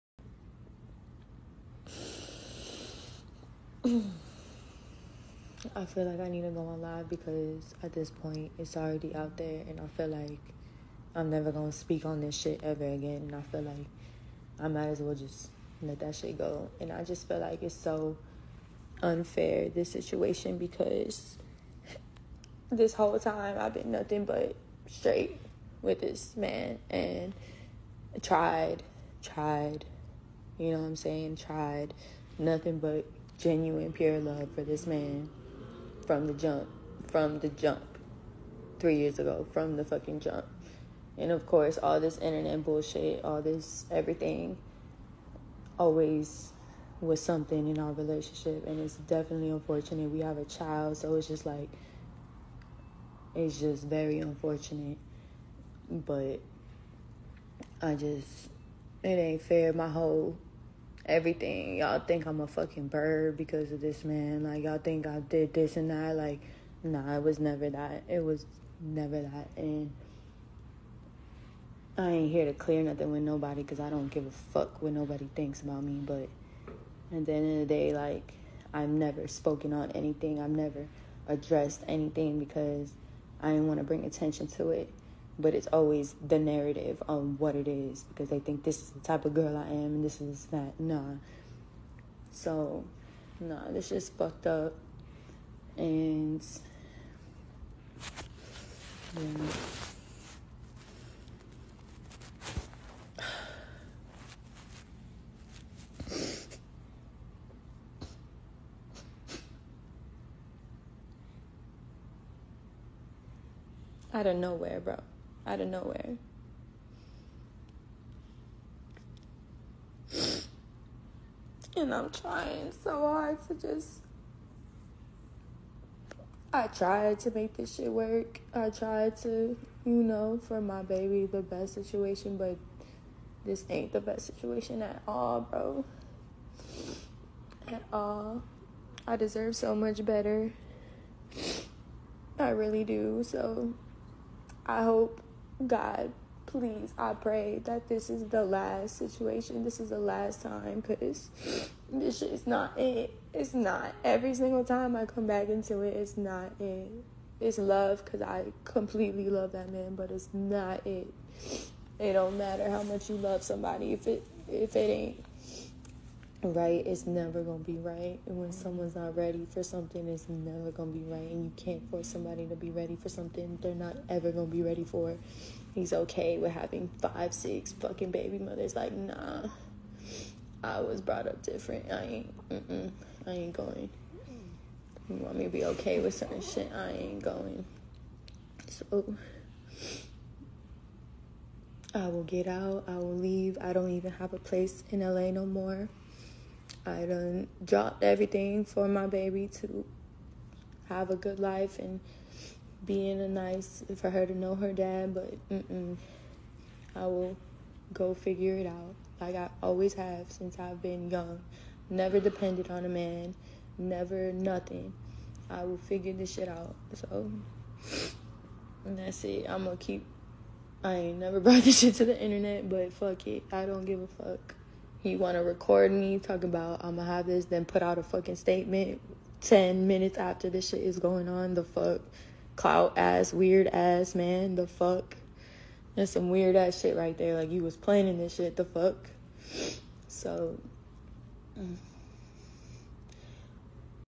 Danileigh on Instagram live crying after the incident with Dababy
Danileigh"I gave Dababy pure love I tried and tried to make happy"